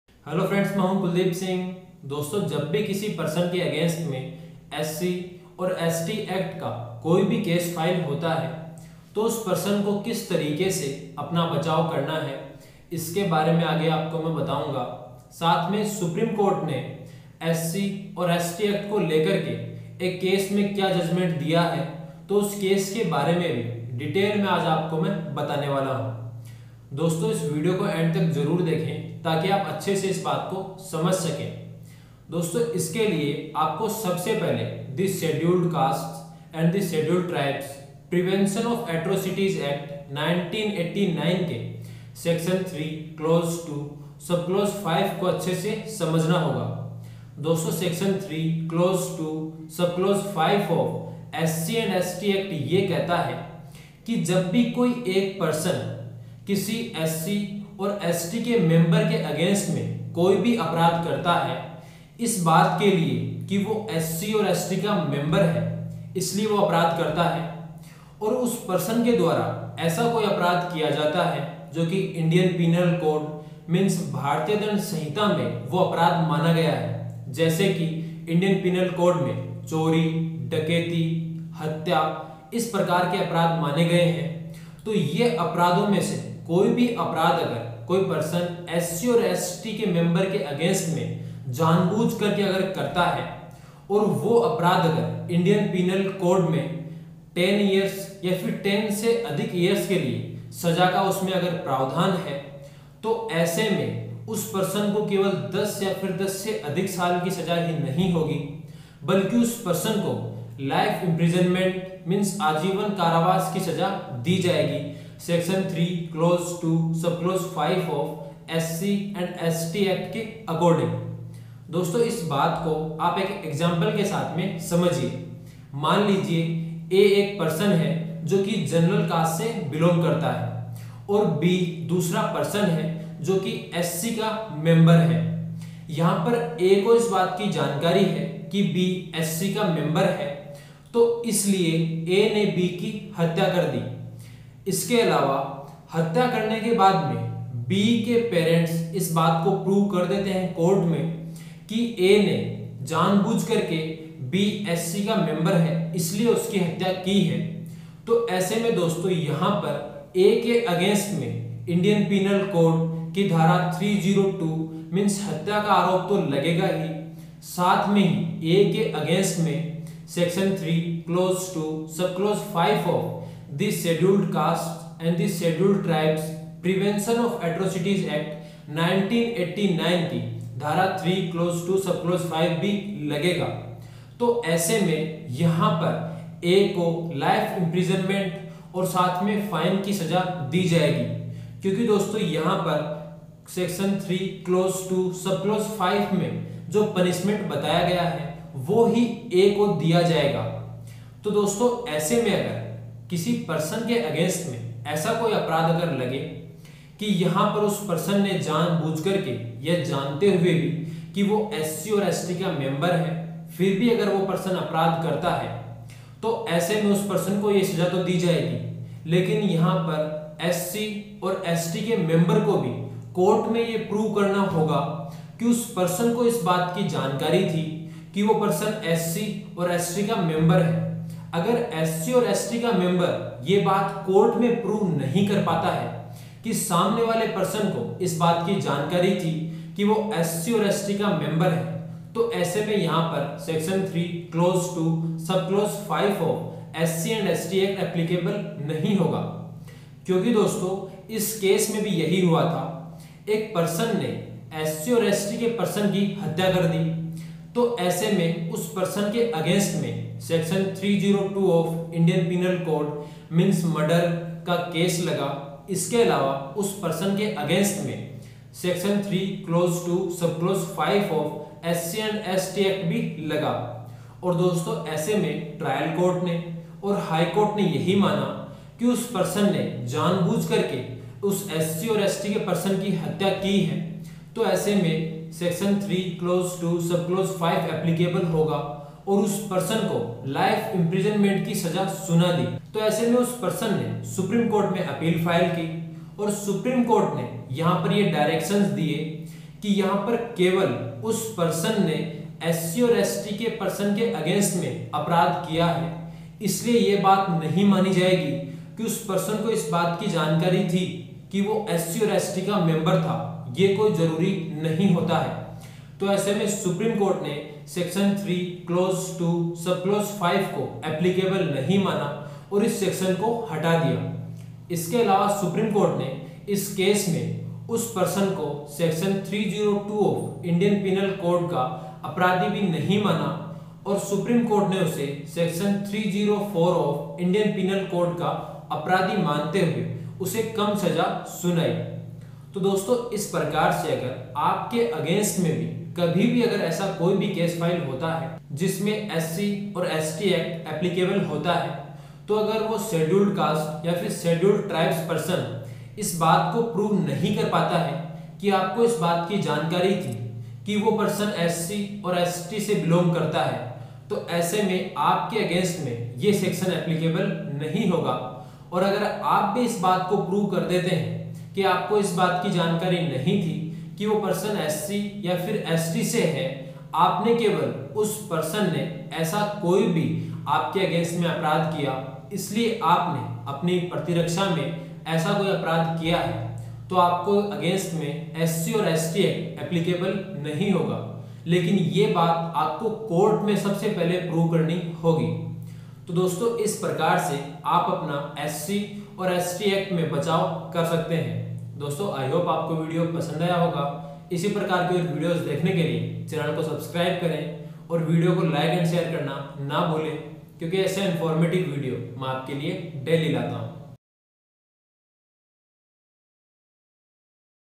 SC & ST Act | अनुसूचित जाति और अनुसूचित जनजाति कानून | Supreme Court Judgment |Case || Kuldeep Singh
Hello friends me hu Kuldeep Singh and is video me mene aap ko 'The Scheduled Castes and Scheduled tribes ( Prevention of Attrocities ) Act, 1989' [ अनुसूचित जाति और अनुसूचित जनजाति कानून ] ke Section 3(2)(v) ke baare me explain kiya hai and Supreme Court ne recently ek case me judgment diya hai uske baare me btaya hai.